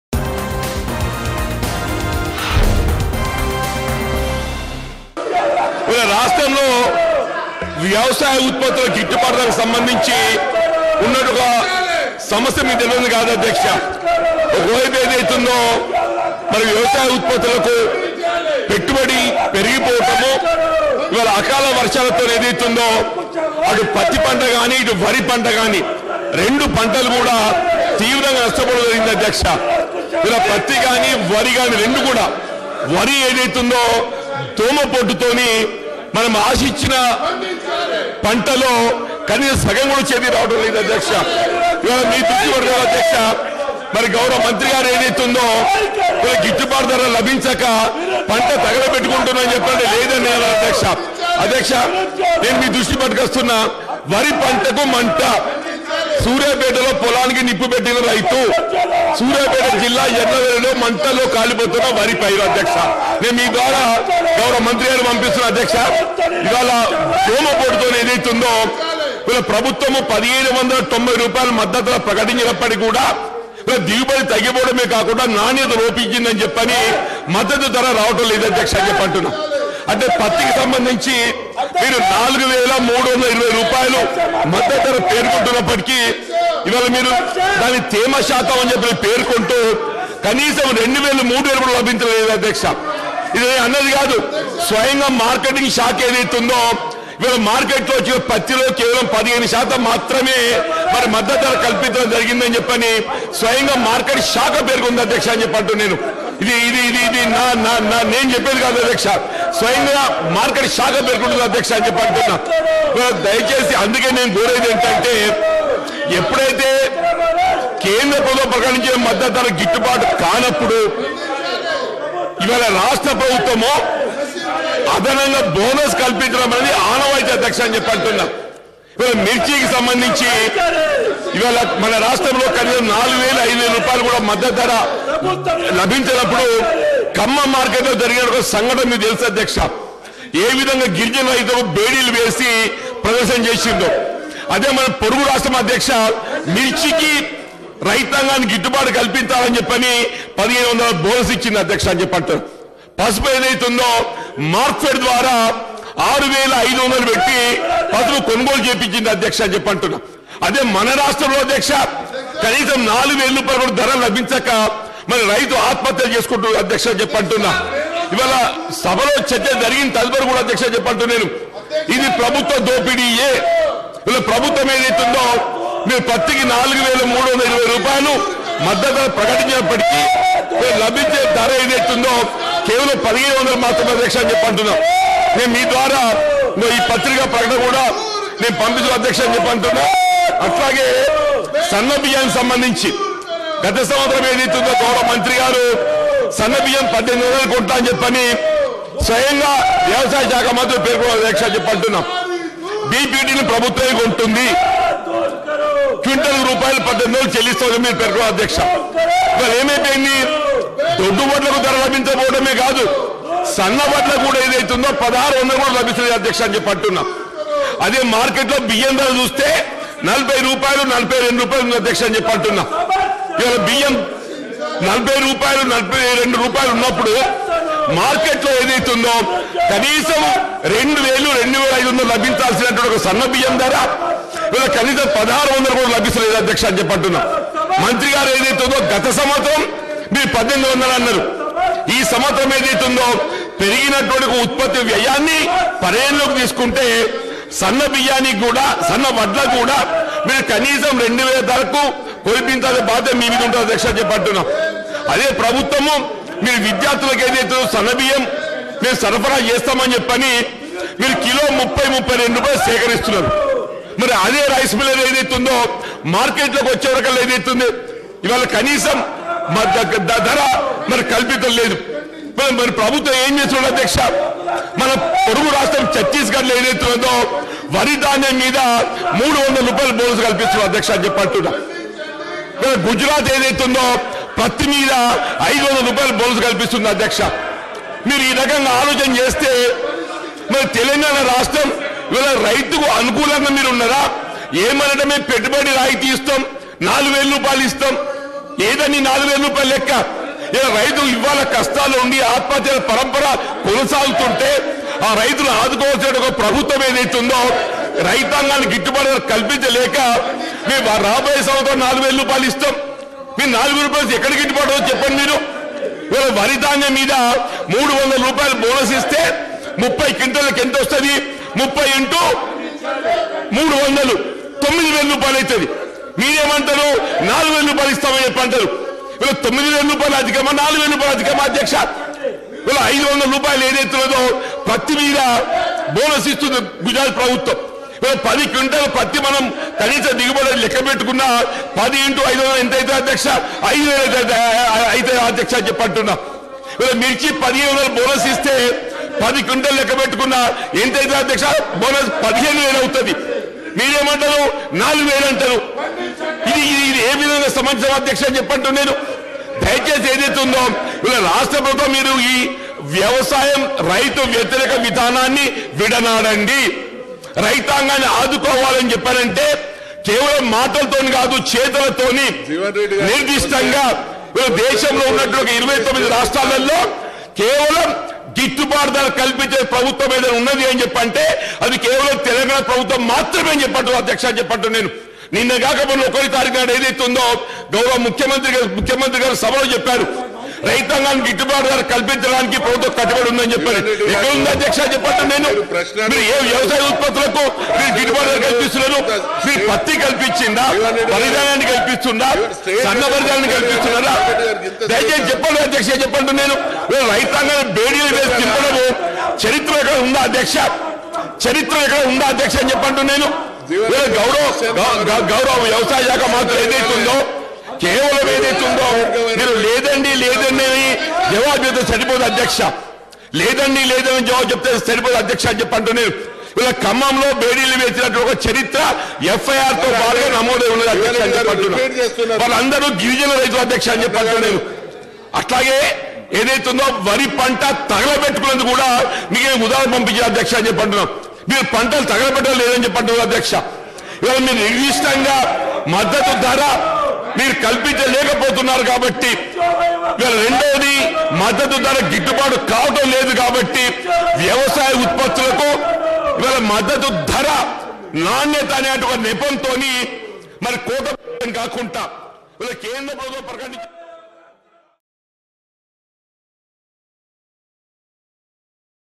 Jeevan Reddy Demands MSP For Seasonal Crops | Telangana Assembly | Mango News
Congress leader Jeevan Reddy demands MSP for seasonal crops and explained on how the production of agricultural investment has increased. He also addressed that cotton crops and rice crops are seriously damaged due Insect Assault  leaves farmers worried.